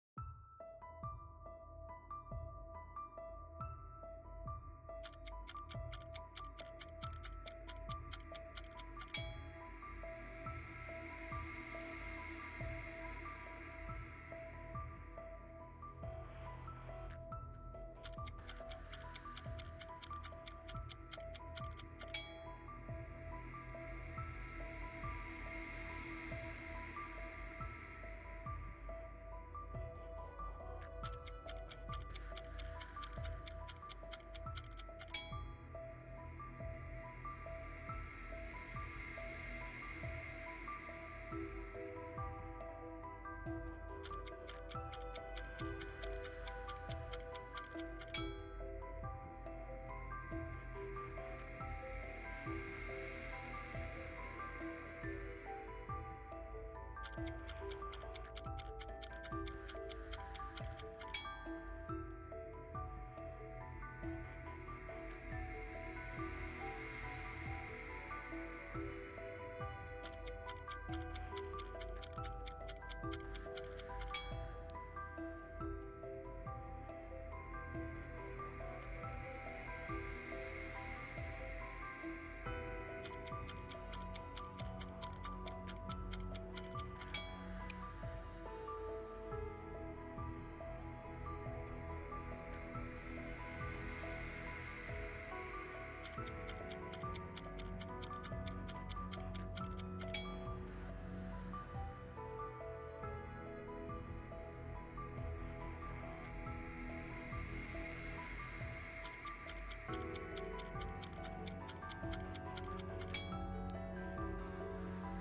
Guess the country name from the flag || 3D Stock Room
welcome to 3D Stock Room channel.

Note:
this data is taken from:

Flags that look alike

Australia and New Zealand
Chad and Romania
Guinea and Mali
Indonesia and Monaco
Ireland and Ivory Coast
Luxembourg vs Netherlands
Norway and Iceland
Venezuela Vs Ecuador Vs Colombia
Bahrain and Qatar